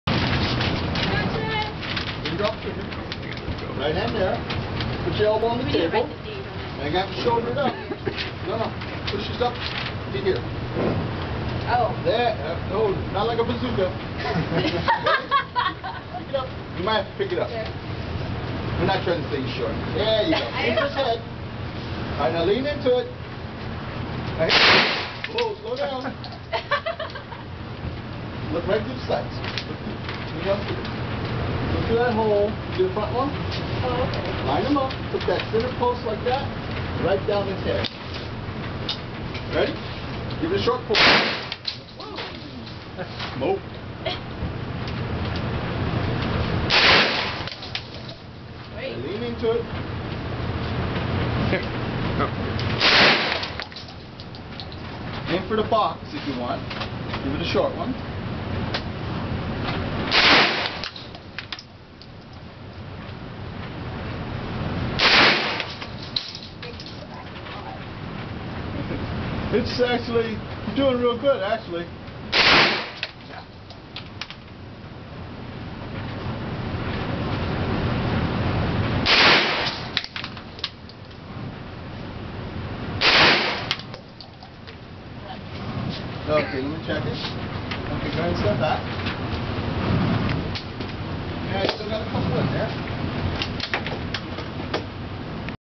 Jessica shoots an MP5
Jessica fires a gun for the first time, and it happens to be a submachine gun.